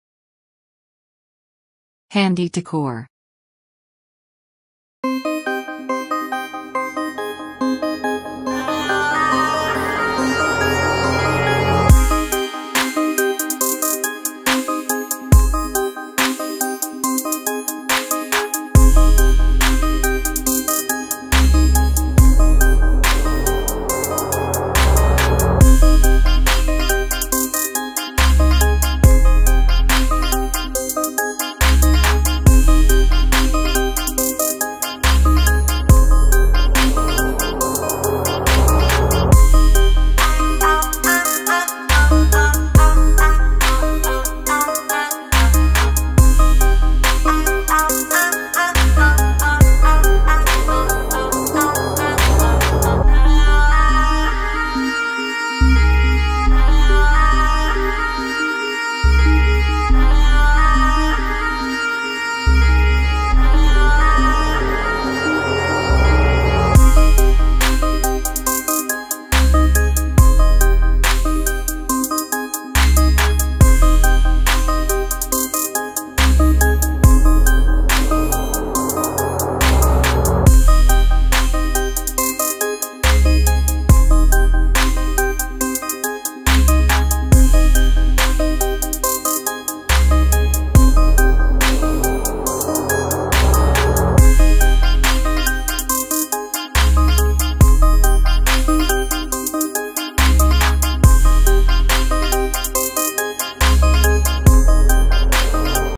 Easy wall hanging diy/Bamboo skewers craft/Barbeque Sticks diy/Bamboo skewers diy
Hello friends, Welcome back to another video. This is shruti here.

About Video

Bamboo skewers craft/Barbeque Sticks diy/Bamboo skewers diy/BBQ sticks diy

Today's video is all about beautiful and easy hand made crafts using bamboo skewers. A easy art anyone can make in no time.

Material used

1. Barbeque Sticks

2. Glue gun